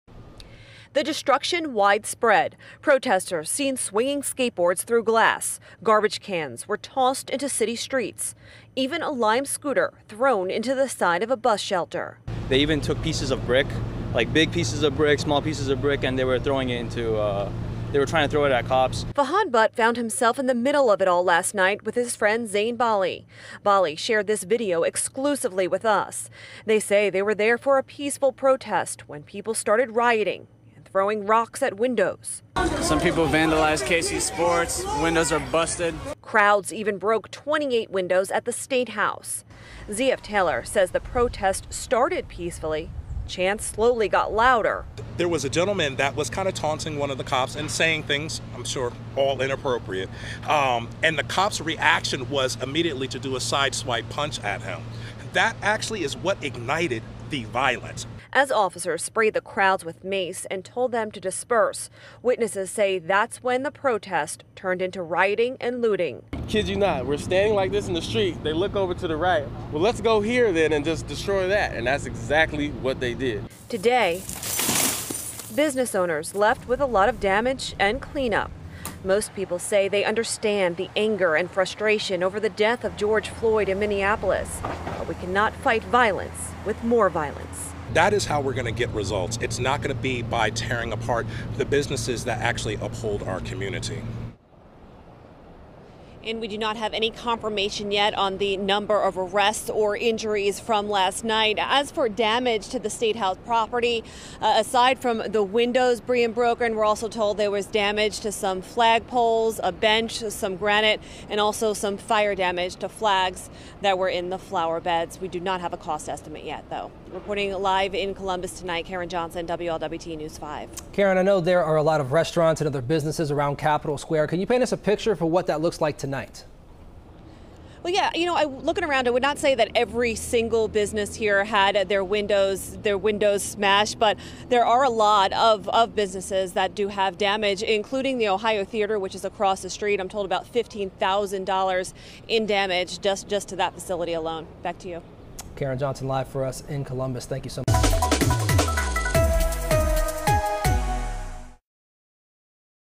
Ohio protests over George Floyd's death turn destructive
Protesters angry over the death of George Floyd in Minneapolis police custody turned out for a demonstration in Columbus that began peacefully but turned destructive, with windows smashed at the Ohio Statehouse and storefronts along surrounding downtown streets.